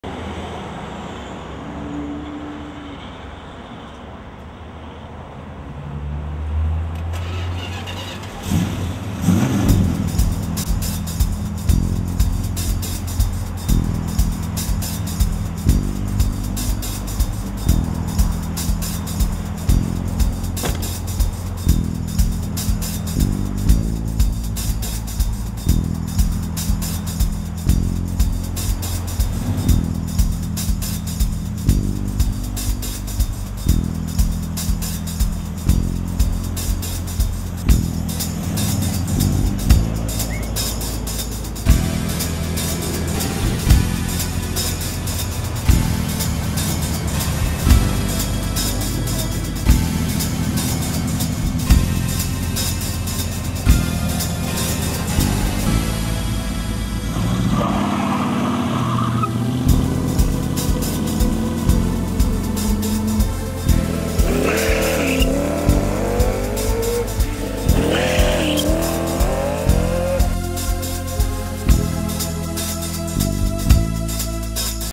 67 CUDA MALIBU
Matt Holland's Barracuda tearin' it up!!!     Music by Cudacade